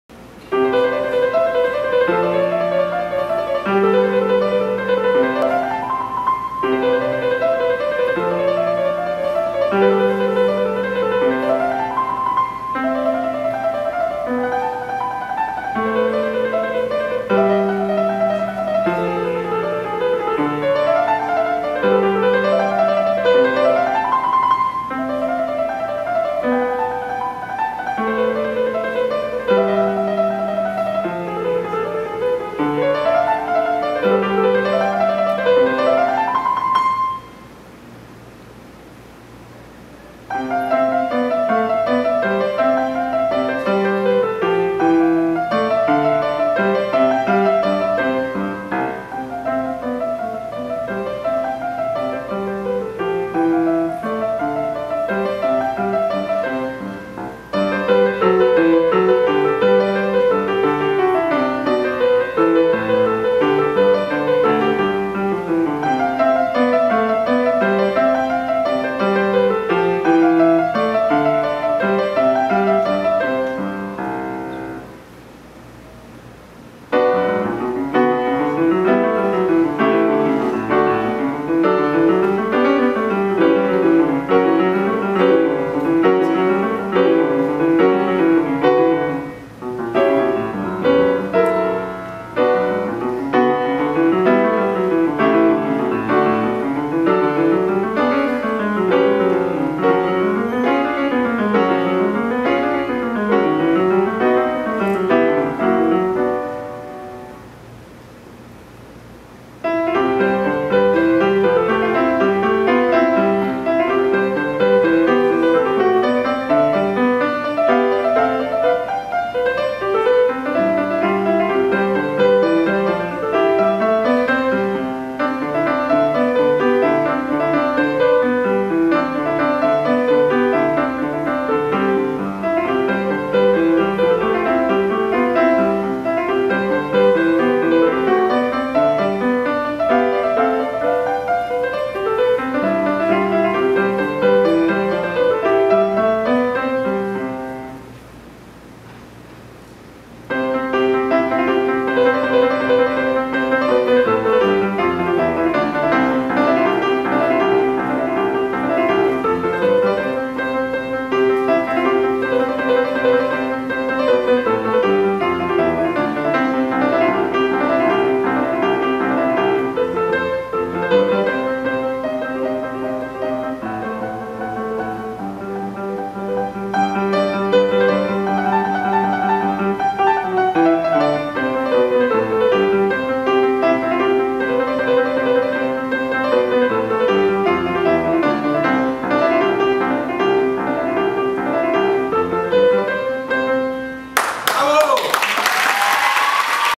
Miguel Hernandez plays at the Festival de Solistas de Venezuela 2019
Esta es la primera participacion de Miguel en el Festival de Solistas de Venezuela. Comenzo a estudiar el piano en Febrero de 2019 y, poco a poco ha brotado su talento. Miguel es de Barinas, Venezuela.